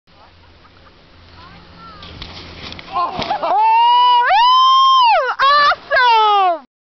Brian's Jump attempt
Brian's 1st and only snowboarding jump